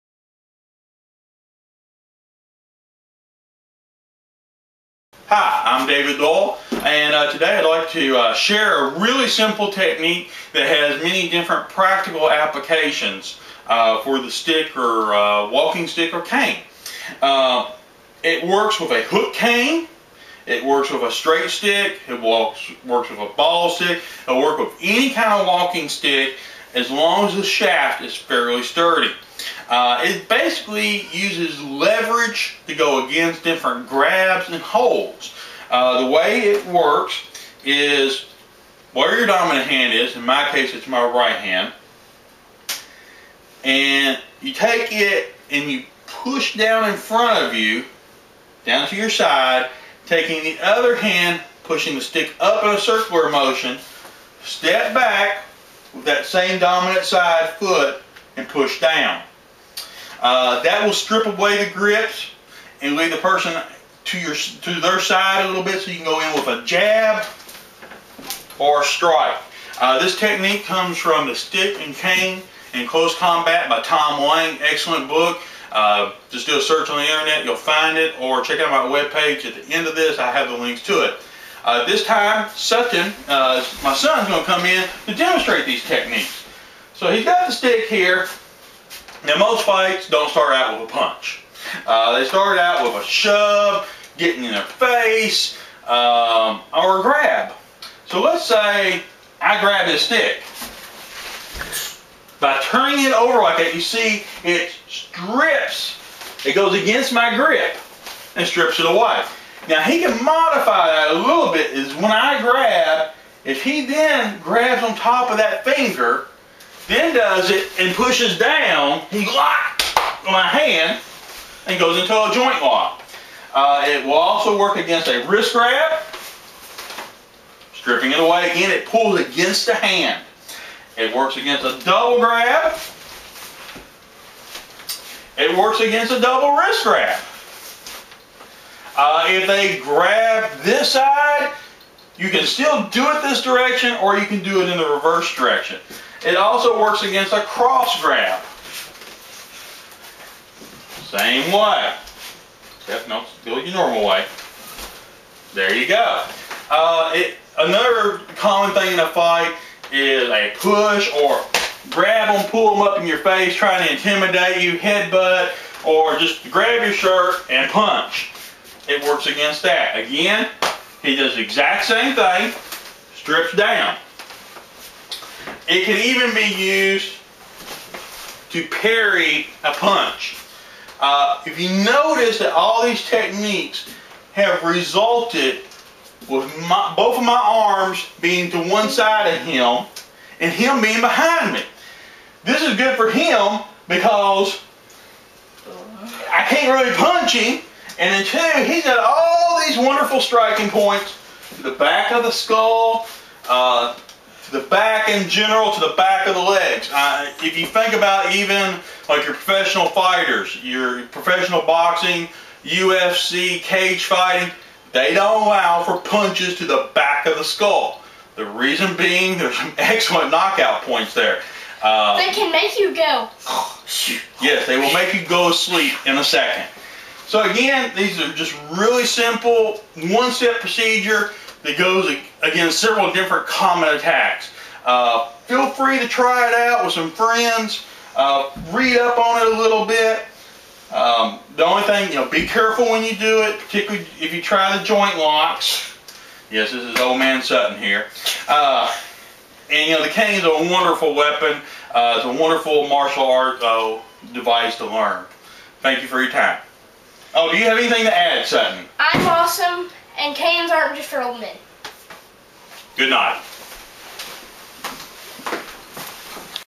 cane walking stick defense against grabs.wmv
Cane / Walking Stick Defense Against Grabs.

This is a video of a simple method that will break holds from various cane, wrist, and lapel grabs.  Based off of the Hip Body Turn from the book "The Stick and Cane in Close Combat" by Tom Lang.